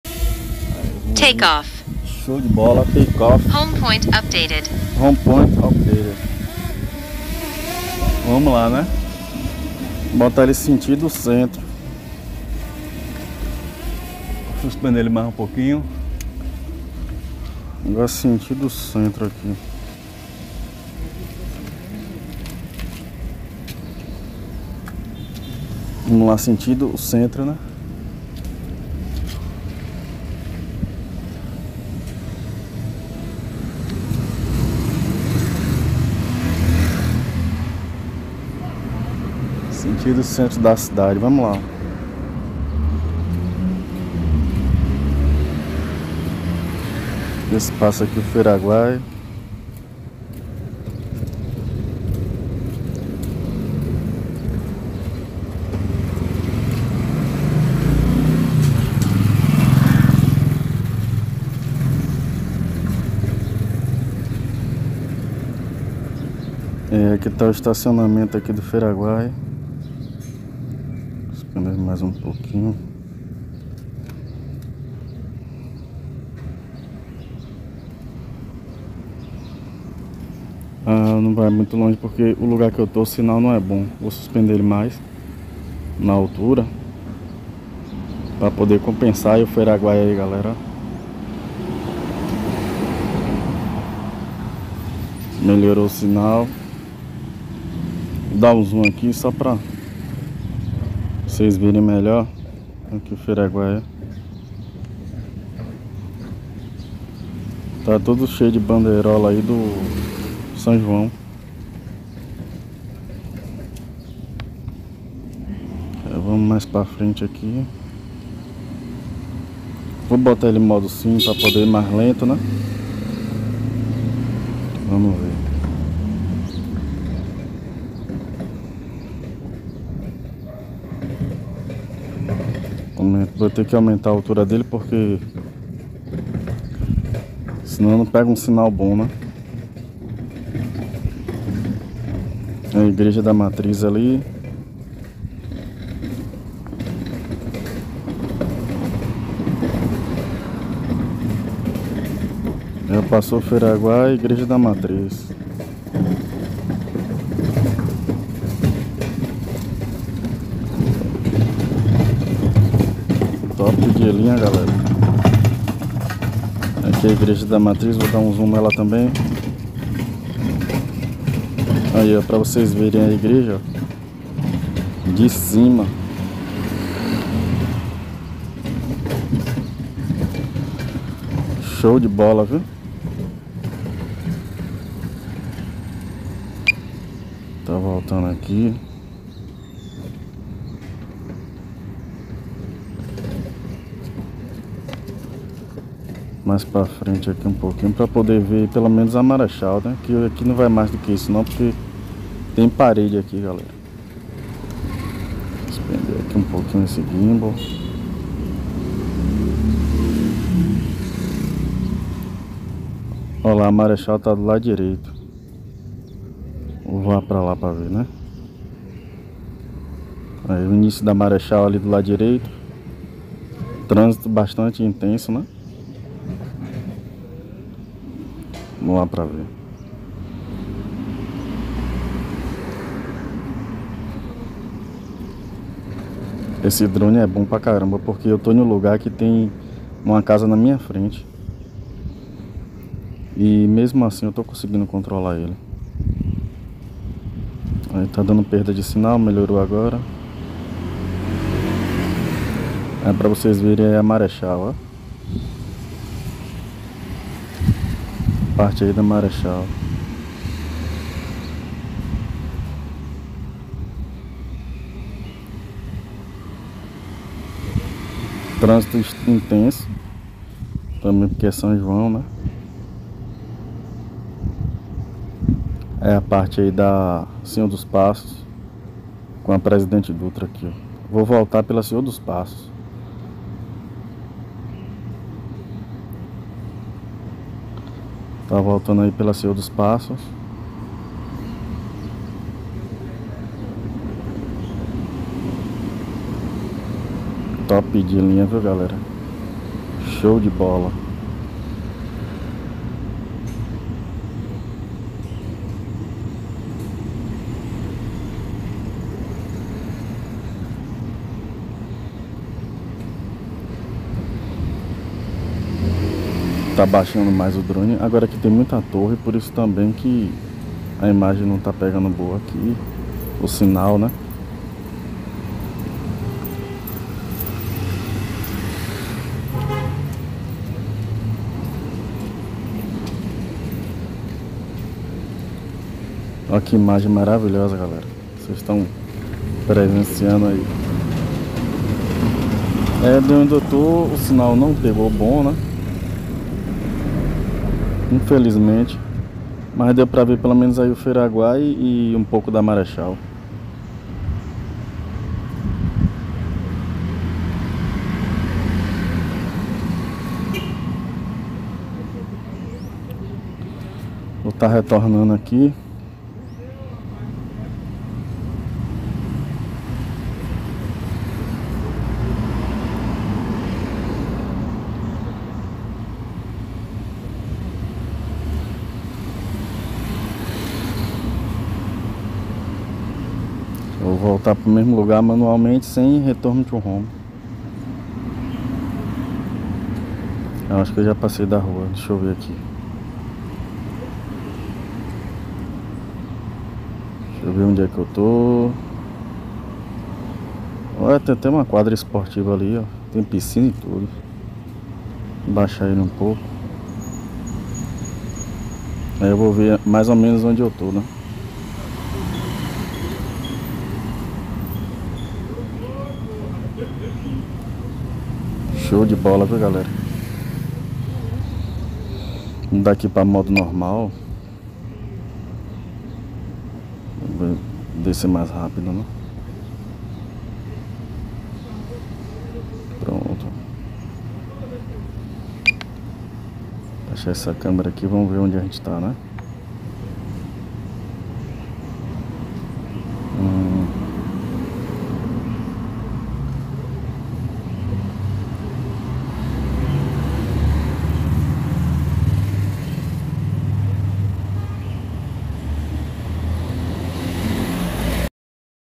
DJI MINI 2SE VOANDO EM FSA CENTRO FEIRAGUAI MARECHAL E IGREJA DA MATRIZ
DRONE DJI MINI 2SE